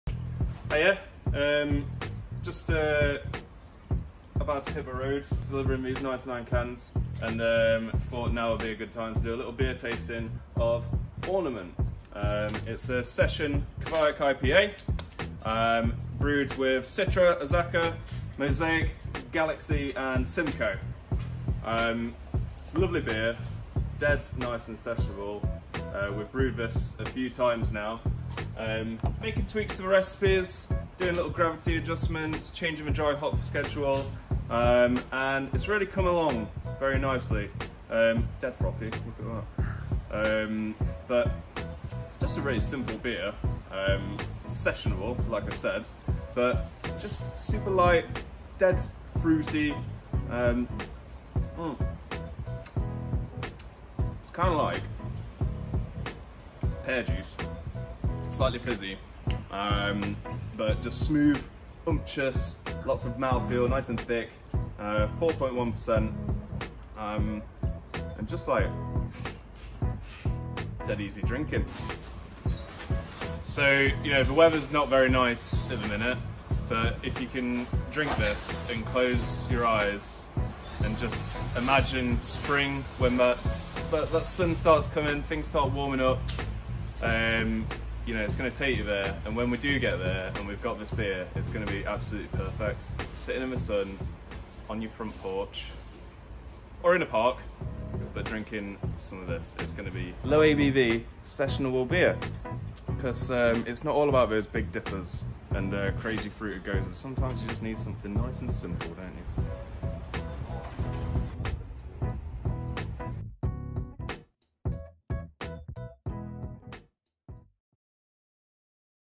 Ornament Session Kveik IPA
Light, refreshing, incredibly smooth and full of fleshy pear flavour; it’s Ornament, our 4.1% Session Kveik IPA. DIPAS, TIPAS AND TFG’s are great, but sometimes you just need something nice and easy to kick back with.